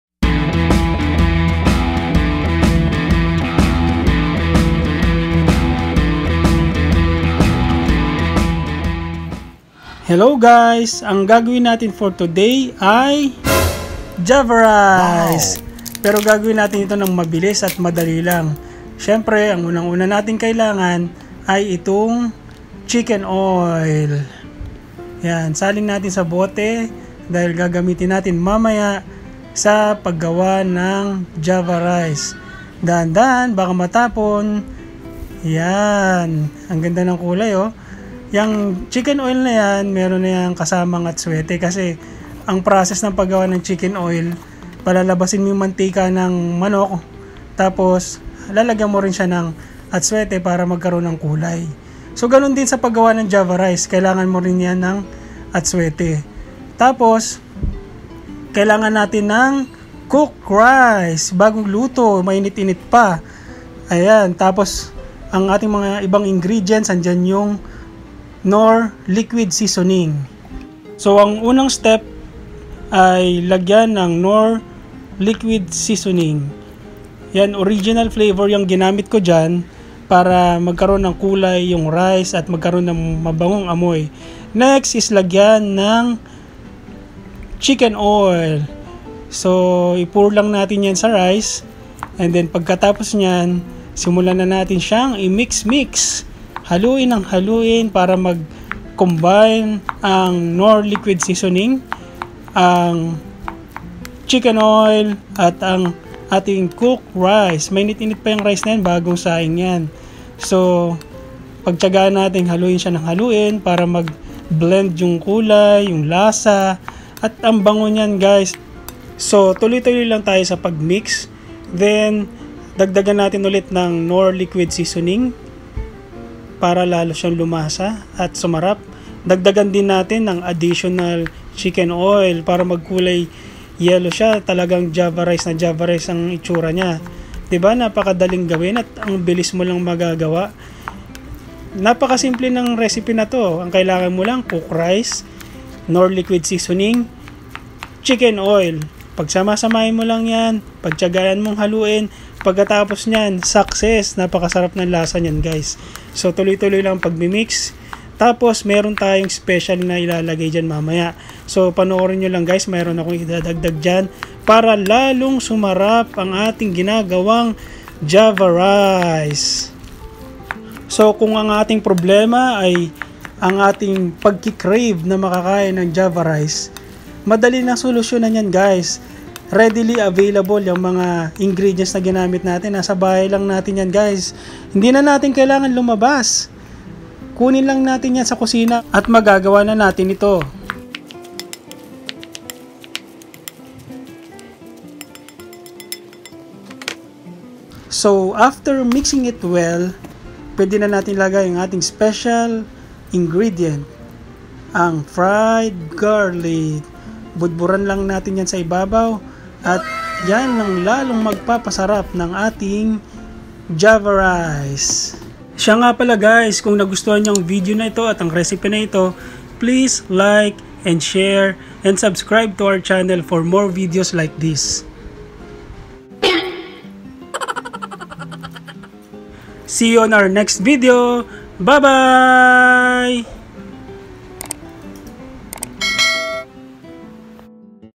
Gawin Mo Rin Ito Sa Morrey Chicken Oil
Tatlong ingredients lang chicken oil plus knorr liquid seasoning plus cooked white rice

Mix well and you will have a java rice of your own

Garnish your rice with fried garlic